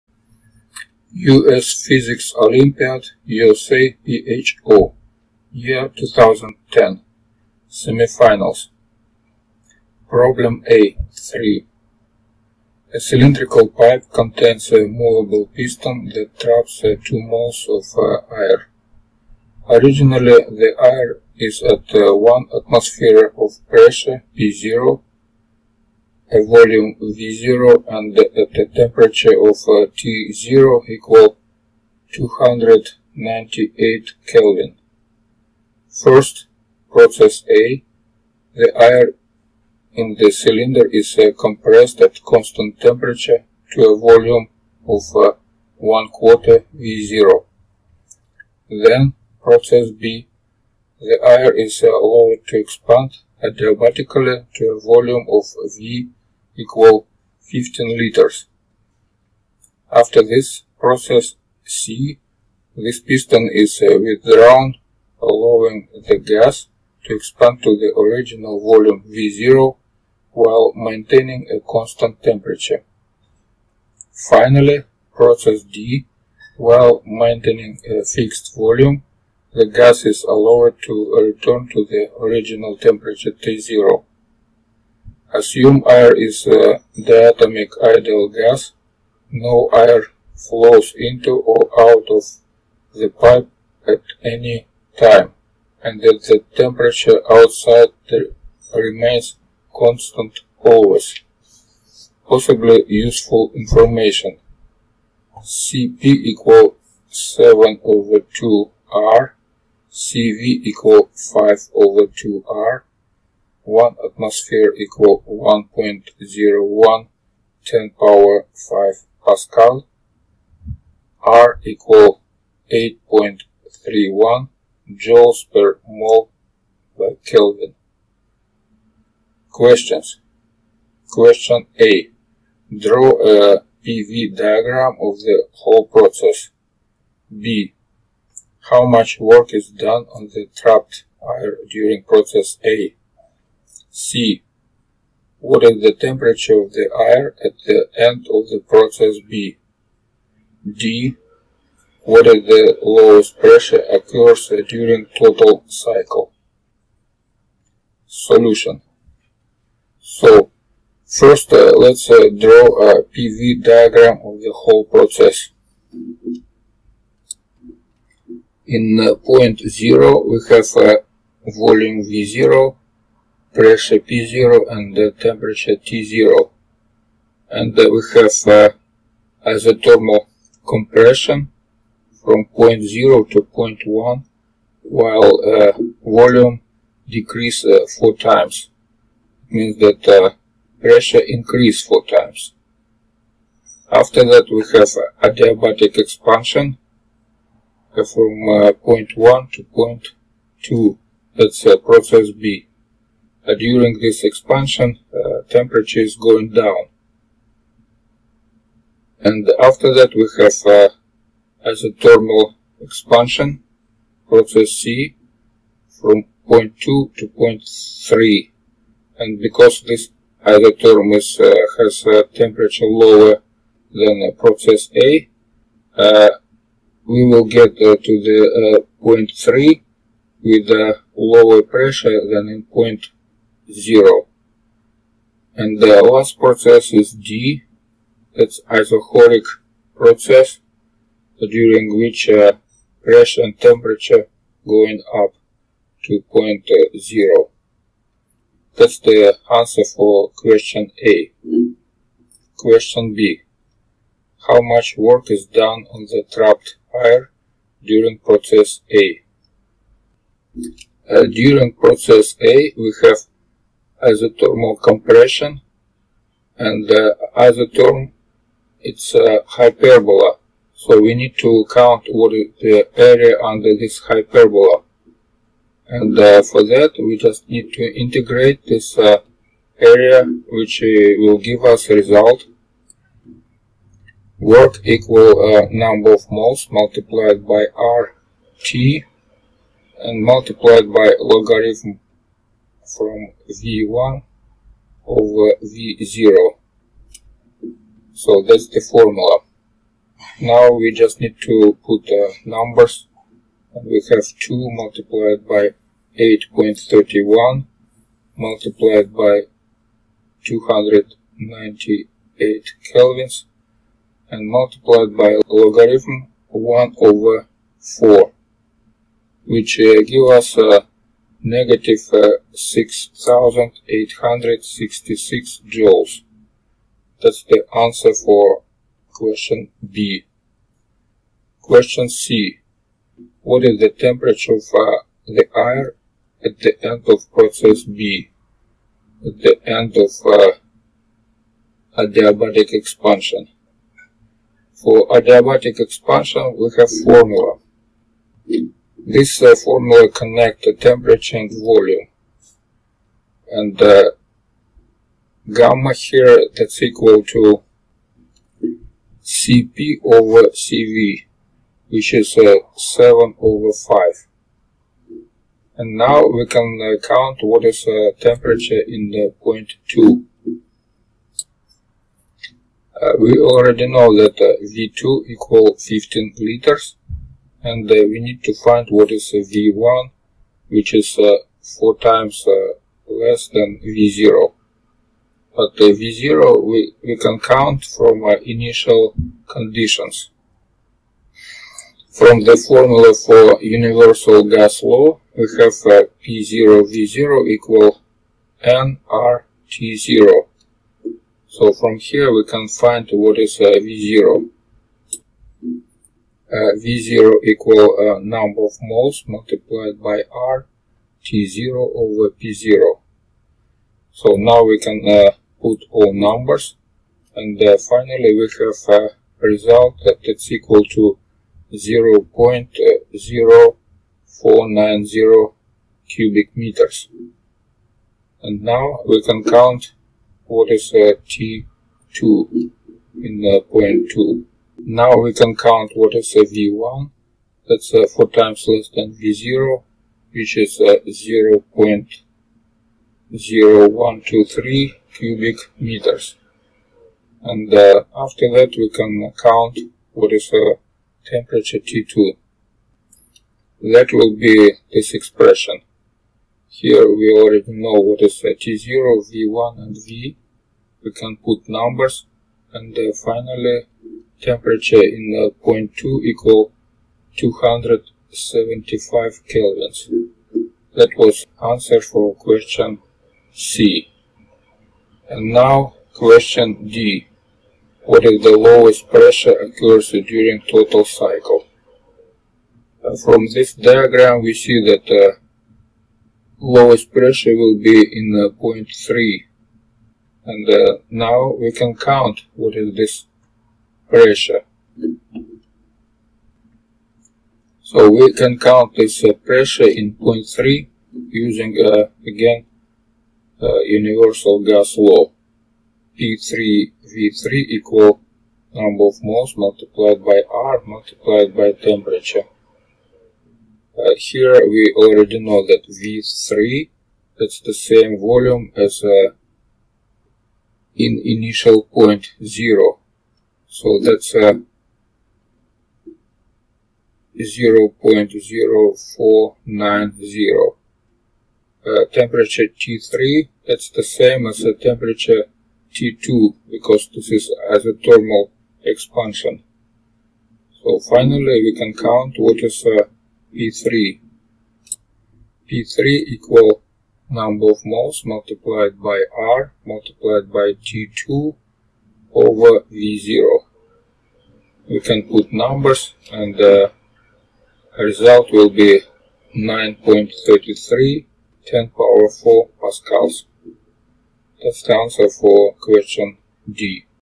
US Physics Olympiad 2010. USAPHO Semifinal Solution for problem A3. Thermo. Gas laws. P-V diagrams.
In this video I presented discussion for  problem A3 from 2010 year Olympiad USAPHO

Solutions
2:37 Question a
4:12 Question b
5:31 Question c
7:54 Question d